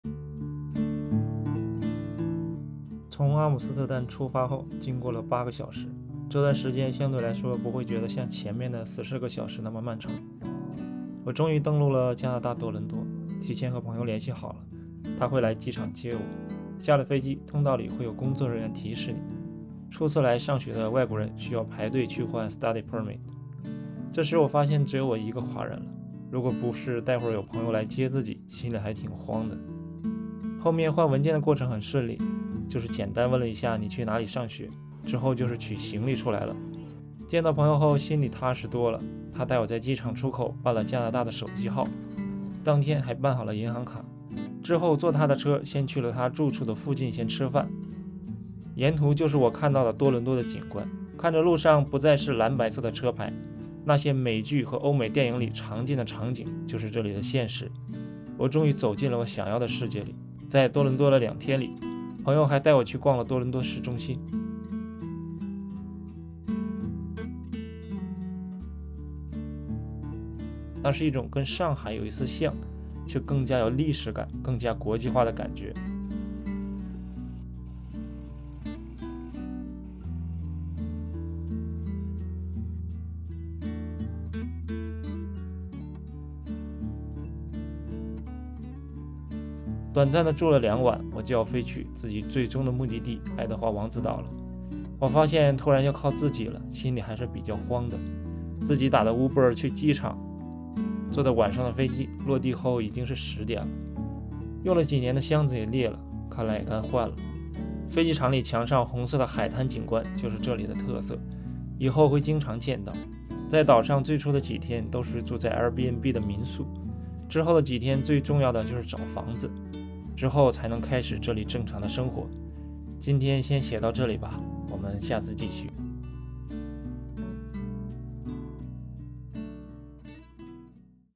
【出国留学移民之路-登陆加拿大】坐了20多个小时飞机，人生第一次登陆加拿大心里慌慌的
从上海坐飞机14小时到达荷兰中转后，又飞了8小时到达多伦多，我见到了一个比上海更大更有历史更现代的国际都市，我也终于走进了我想要的世界里